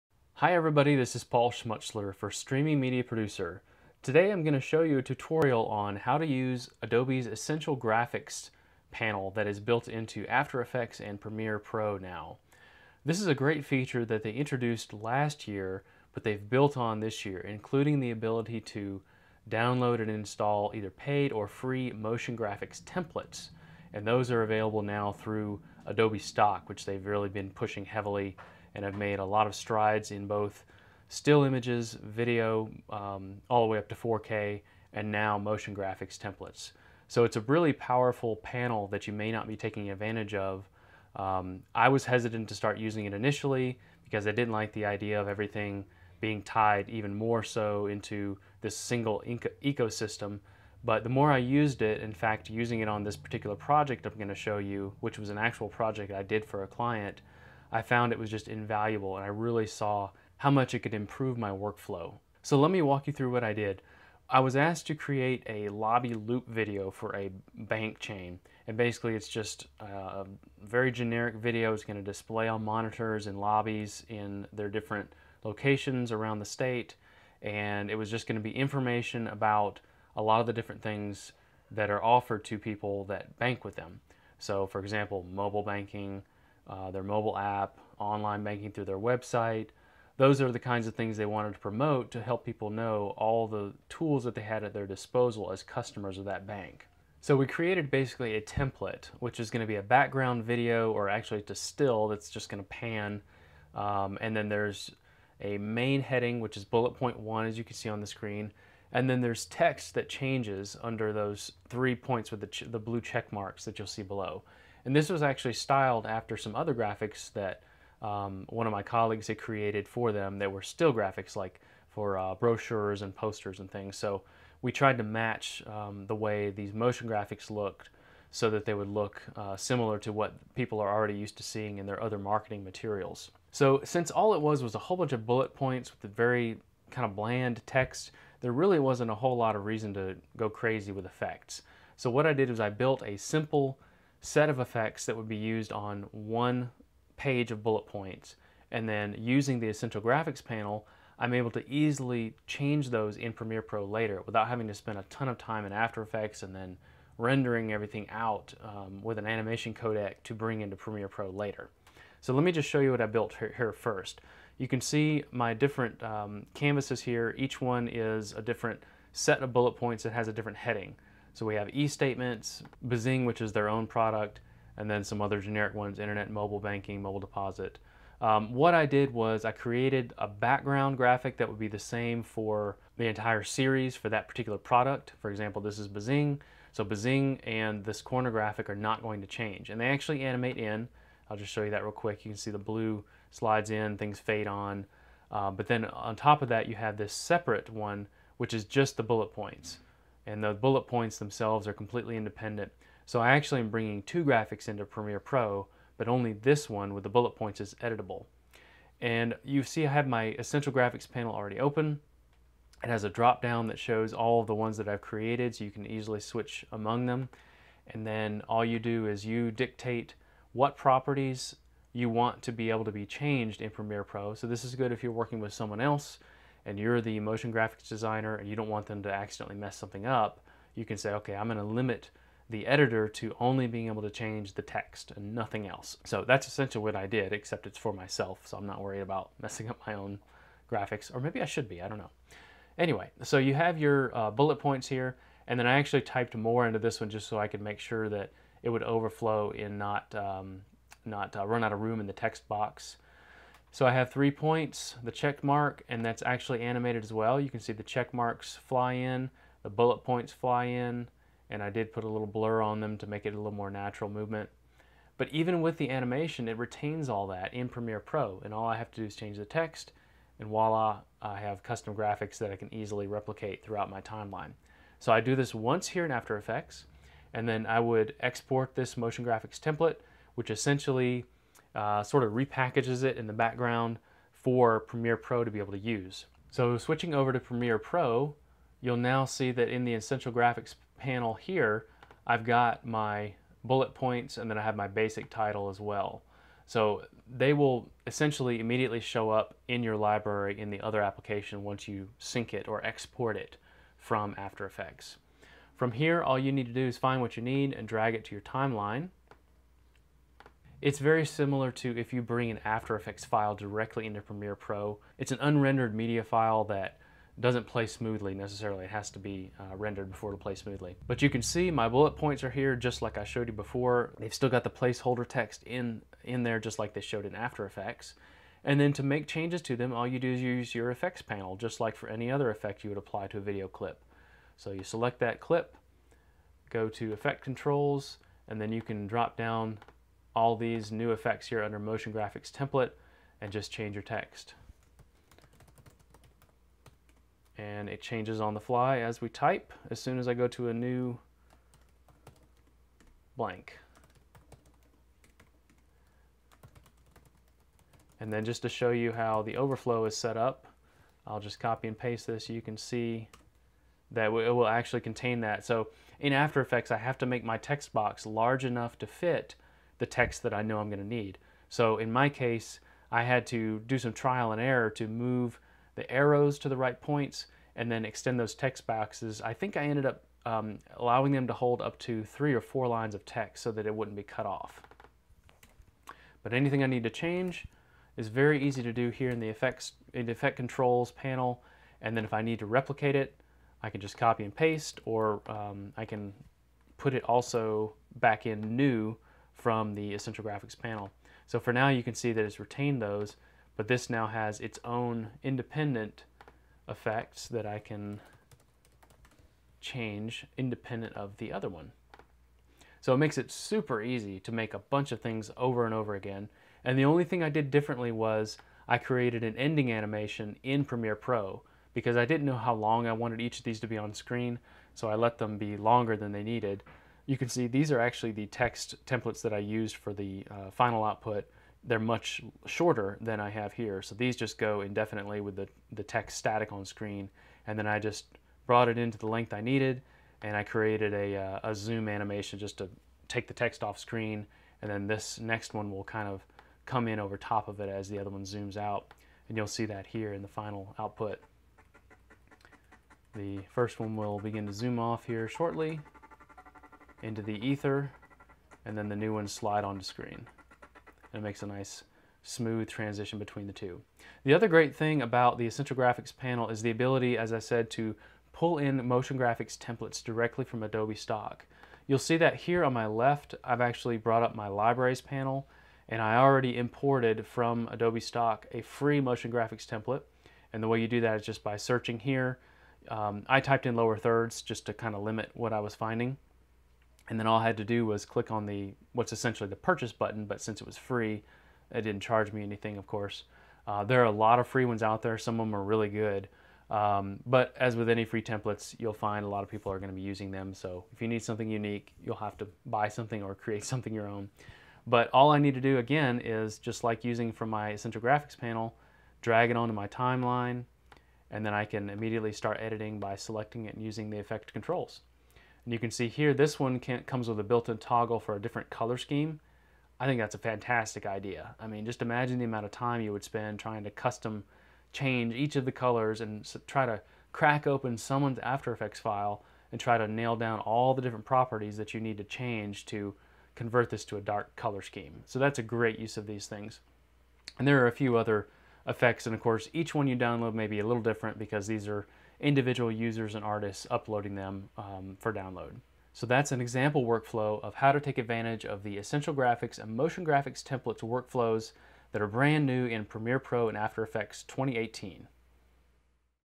Tutorial: Adobe Essential Graphics for After Effects and Premiere Pro
This tutorial will show you how to use the Essential Graphics panel that is now built into Adobe After Effects and Premiere Pro, which now features the ability to download and install either paid or free motion graphics templates.